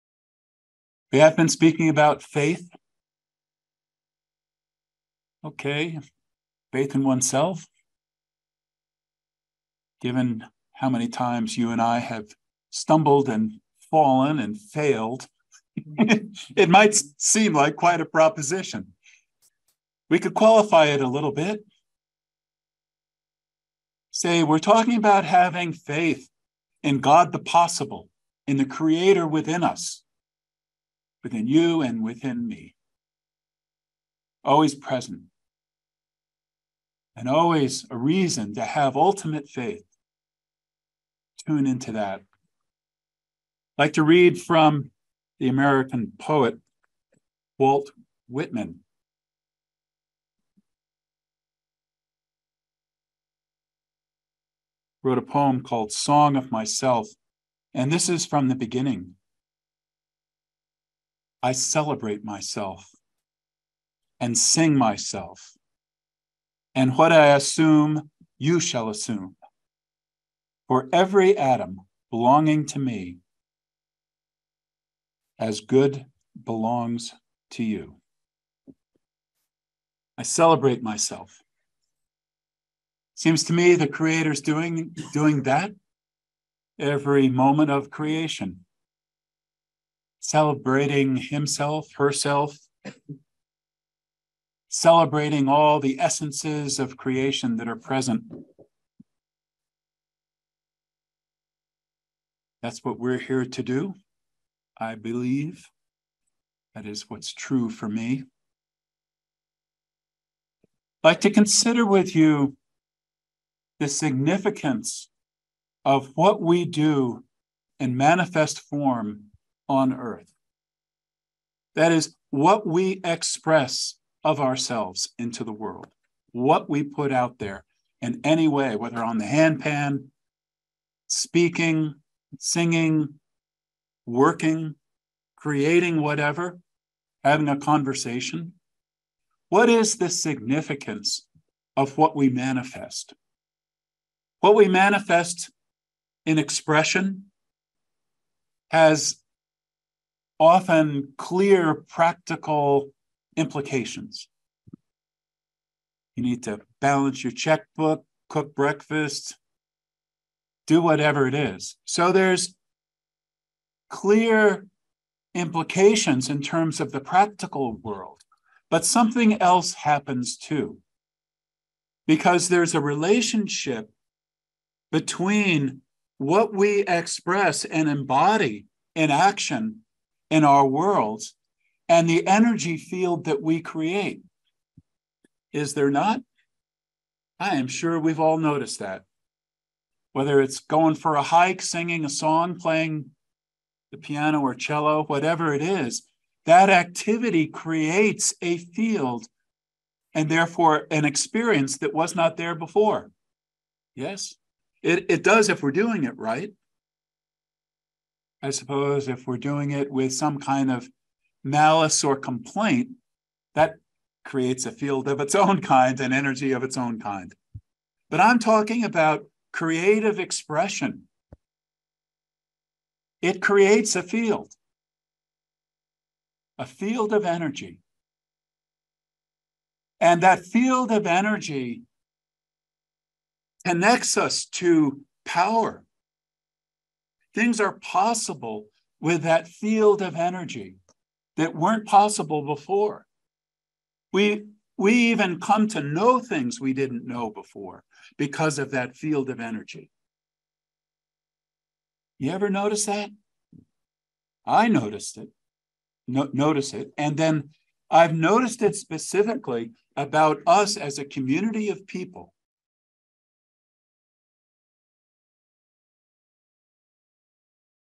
David Karchere: Faith in Oneself
We have been speaking about faith and faith in oneself. We are talking about having faith in God the possible in the creator within us, with you and within me. Always present and always a reason to have ultimate faith...

Spiritual Director David Karchere:  "Faith in Oneself " excerpt from our Sunday service on July 9th, 2023  at Sunrise Ranch in Loveland, Colorado.